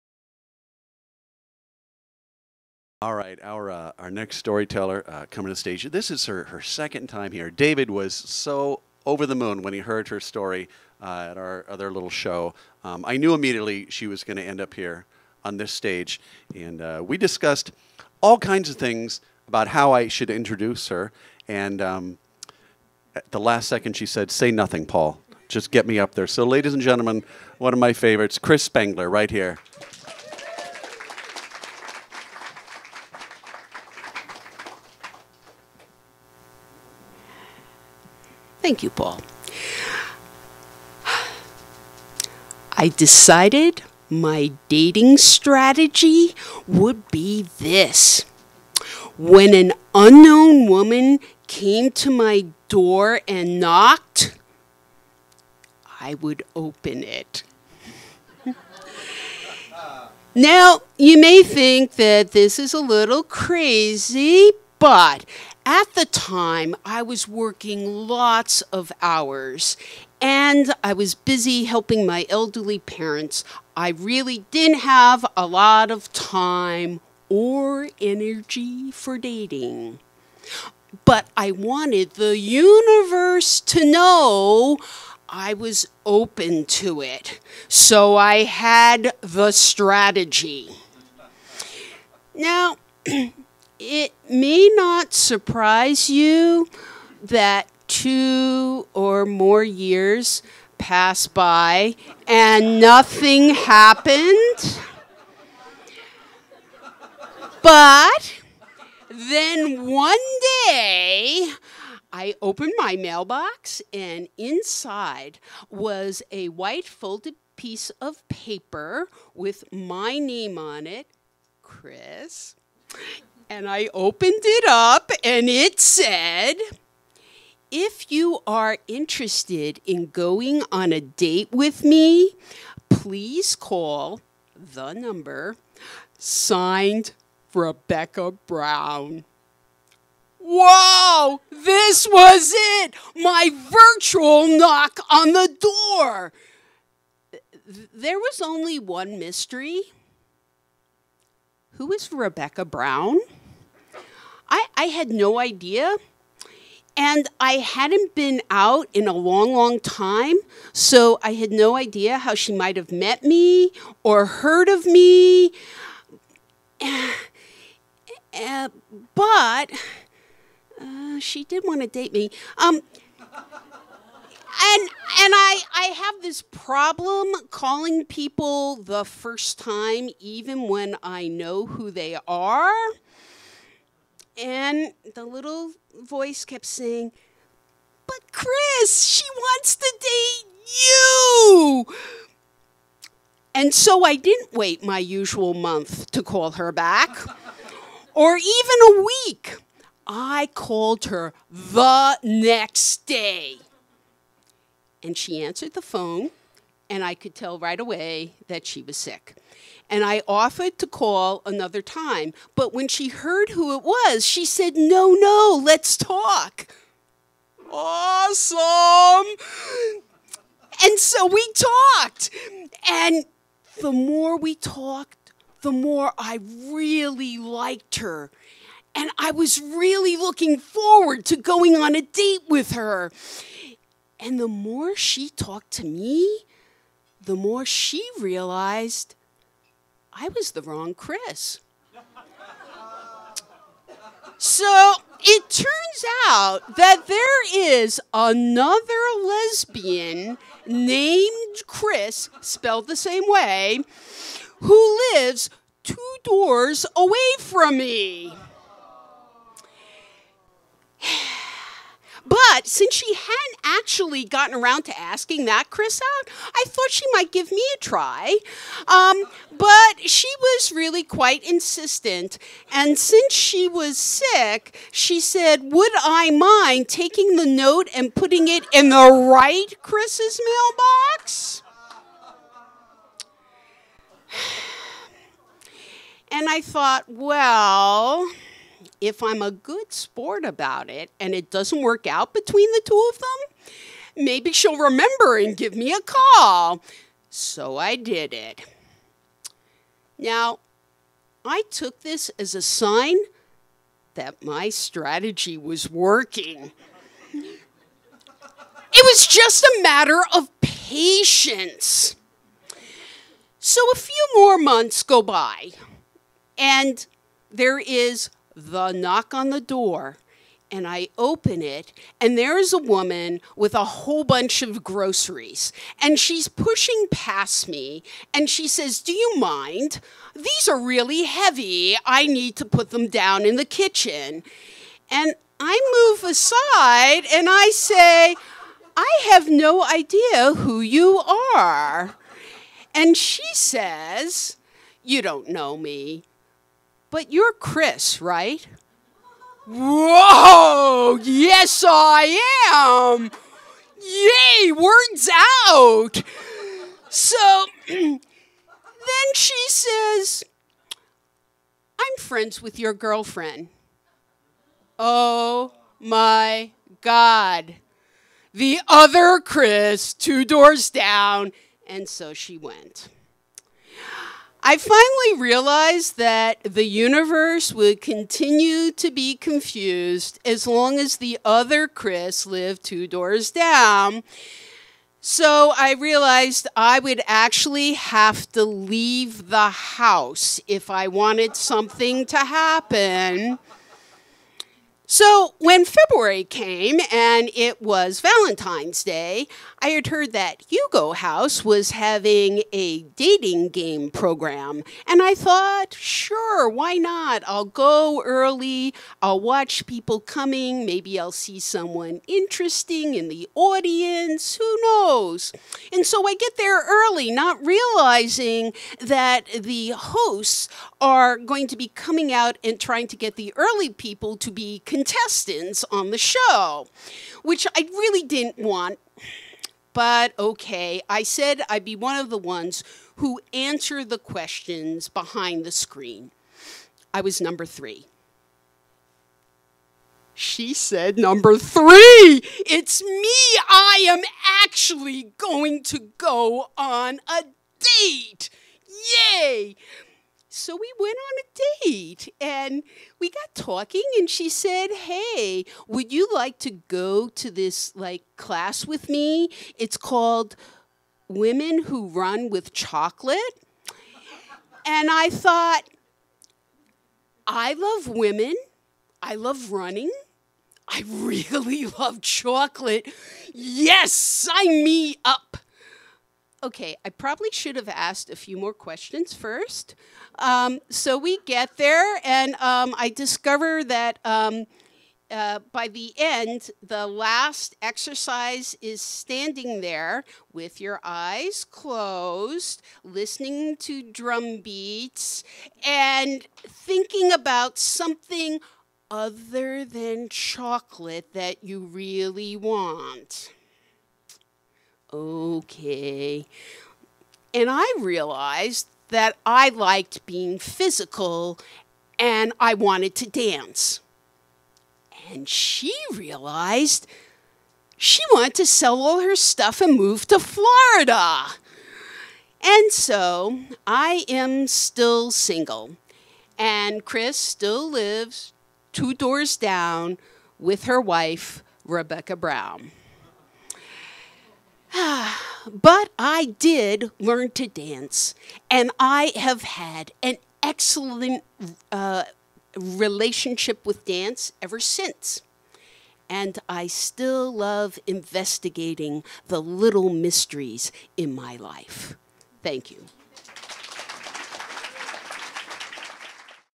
Chris Spengler performs at the Rendezvous for Seattle Words and Music
Chris Spengler performs at the Rendezvous for Seattle Words and Music 04DEC16